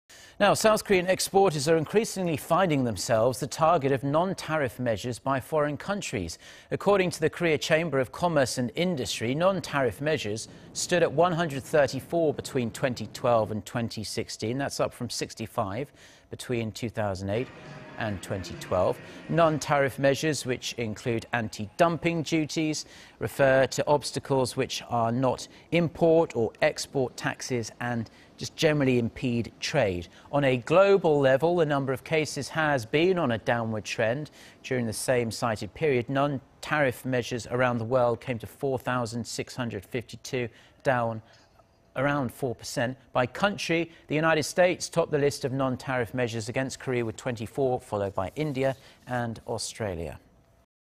Korea slapped with increased non-tariff measures
한국 겨냥 비관세장벽 최근 4년간 2배로 늘었다
Korean exporters are increasingly finding themselves the target of non-tariff measures by foreign countries.
According to the Korea Chamber of Commerce and Industry,.. non-tariff measures stood at 134 between 2012 and 2016,... up from 65 between 2008 and 2012.
Non-tariff measures, which include anti-dumping duties, refer to obstacles which are not import or export taxes,... and impede trade.
On a global level,... the number of cases has been on a downward trend.
During the cited period,... non-tariff measures around the world came to four-thousand-652,... down almost four percent.
By country,... the U.S. topped the list of non-tariff measures against Korea with 24, followed by India and Australia.